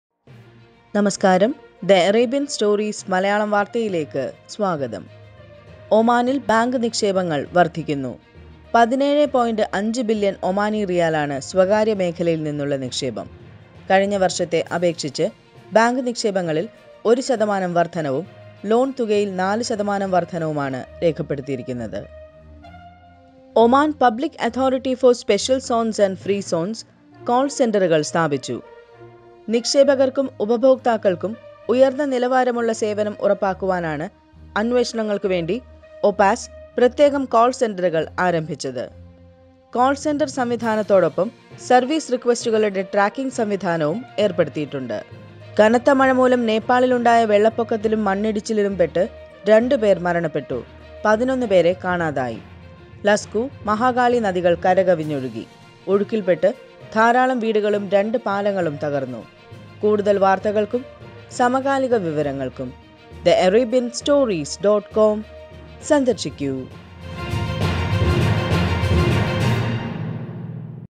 TAS Malayalam News: ഒമാനിൽ ബാങ്ക് നിക്ഷേപം വർധിക്കുന്നു| The Arabian Stories
ഒമാനിൽ ബാങ്ക് നിക്ഷേപം വർധിക്കുന്നു

OPAZ ഒമാനിൽ കാൾ സെന്ററുകൾ ആരംഭിച്ചു

നേപ്പാൾ വെള്ളപ്പൊക്കം; 2 മരണം

Powered by: NMC Healthcare Oman